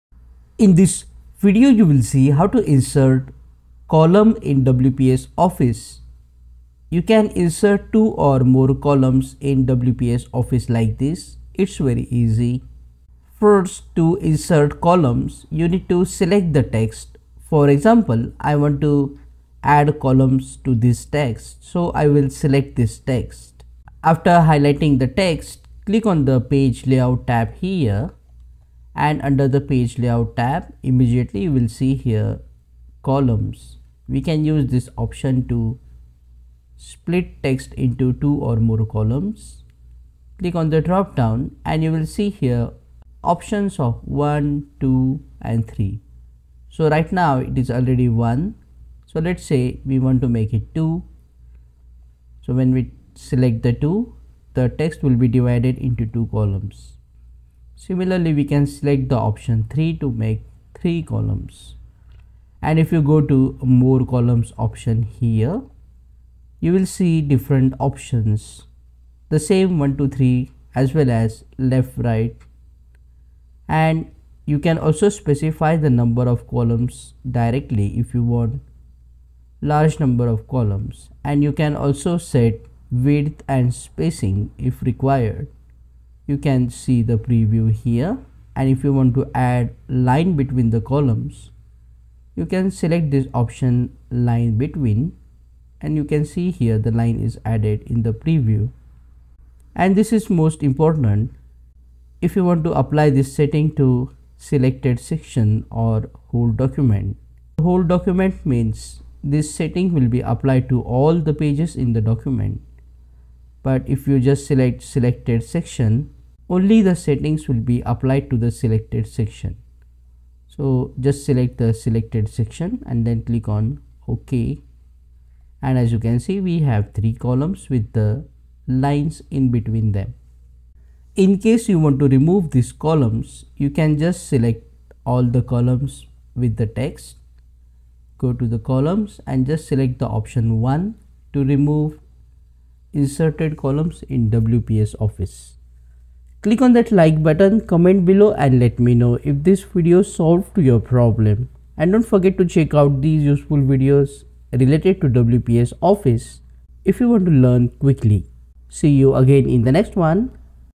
How To Insert Column In WPS Office
How To Insert Column In WPS Office Writer is shown in the video. You can split the document and make two, three or more columns with and without lines in between them in the document